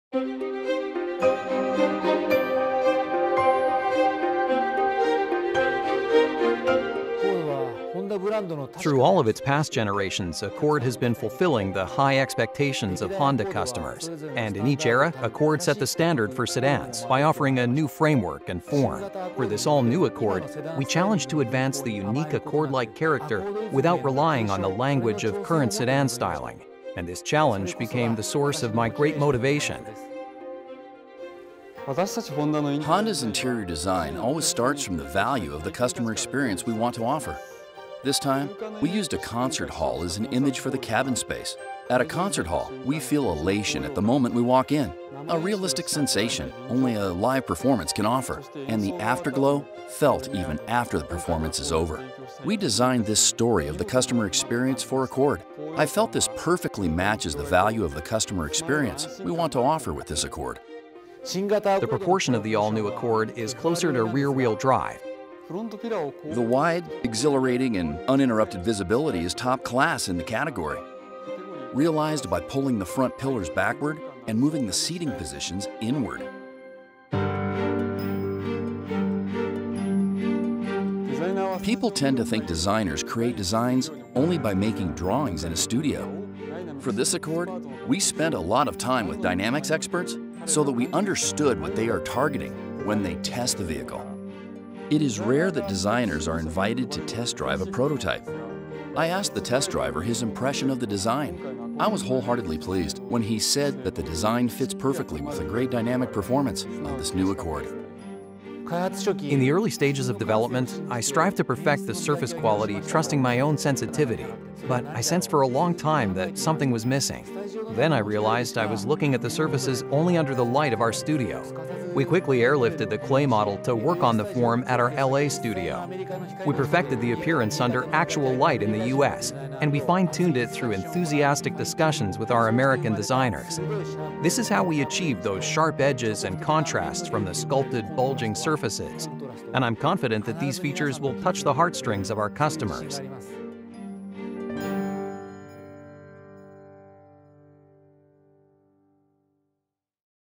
2018 Honda Accord (US Spec)
Preliminary specifications:
Length/width/height/wheelbase - 192.1/73.2/57.1/111.4 in (4,879/1,859/1,450/2,829 mm)
Engine variants:
* 1.5T
Engine - 1.5-liter (1,498 cc), 4-cylinder turbo petrol
Max torque - 192 lb-ft (260 Nm) at 1,500-5,000 rpm
Transmission - 6-speed manual / continuously variable (CVT); FWD
* 2.0T
Engine - 2.0-liter (1,996 cc), 4-cylinder turbo petrol
Max torque - 273 lb-ft (370 Nm) at 1,500-4,000 rpm
Transmission - 6-speed manual / 10-speed automatic; FWD
* Hybrid
Petrol engine - 2.0-liter (1,993 cc), 4-cylinder petrol
Max output - TBA
Max torque - TBA
Transmission - Electric continuously variable (E-CVT); FWD